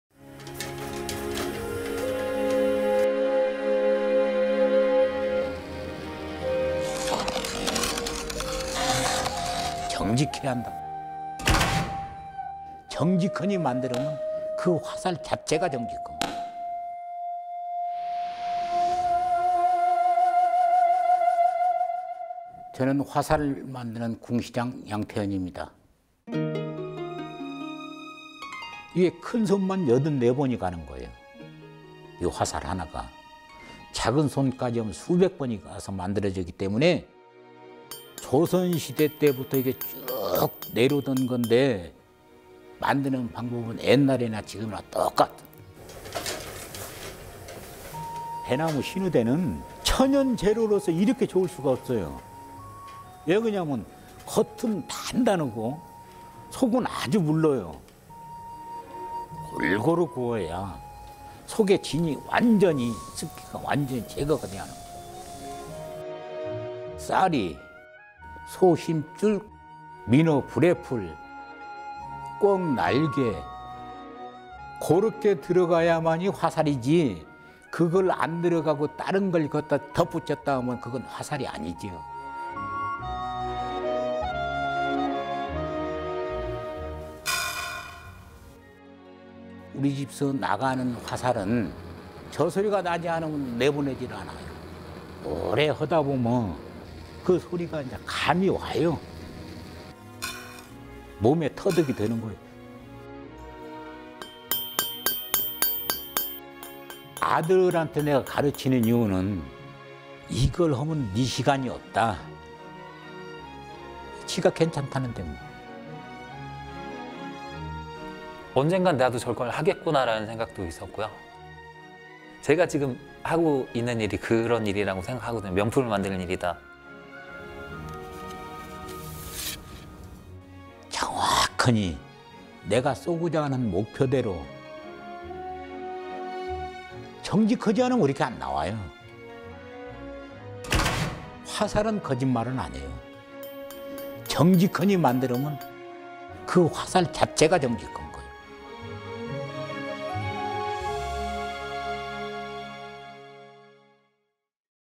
The Honest Arrow, Korean Culture, Timeless Values
Timeless Values, The Honest Arrow

Yang Tae hyun/ Important
Intangible Cultural Asset No. 16, the bowyer

"You must be precise.
If you make it with precision,
then that arrow will be accurate."

Cheongju,
Chungbuk Province, South Korea

There are 84 important steps that go into making one basic arrow.

To make a more detailed arrow, there are hundreds of steps involved.

This method of making arrows was passed down from the Joseon Dynasty.

The way we make arrows now, is the same as the way they were made long ago.

There’s no better material found in nature than Shinoodae bamboo.

That’s because the exterior is hard and the inside is very malleable.

You have to heat treat the bamboo to completely rid it of all humidity and sap.

Bamboo
Bush clover
Cow tendon
Croaker’s air bladder
Pheasant wings

These materials must go into the making of an arrow.

If they don't, or are replace with other materials,

then what you have isn’t a true arrow.

The arrows we sell here must make this noise.

If they don’t, we don’t sell them.

After all my years of experience, I can tell how good an arrow is by it’s sound.

It’s like second nature to me.

My son is willing to devote himself to this all time consuming job.

That’s why I teach him this craft.

Yang Chang Eon / Important Intangible
Cultural Asset No. 16, bowyer apprenticeship graduate

I always thought of doing this kind of work one day.

I equate the job I do now, to something in the lines of making luxury goods.

You have to make the arrow with care and precision, so that it hits the target.

If your craftsmanship isn’t precise, that arrow won’t hit the target.

Arrows don’t lie.

If you make it with precision, that arrow will be accurate.

Directed by Daewoong Kim
Cinematography by Byoungsoo Kim
                  Hyunki Lee
ⓒMBC Chungbuk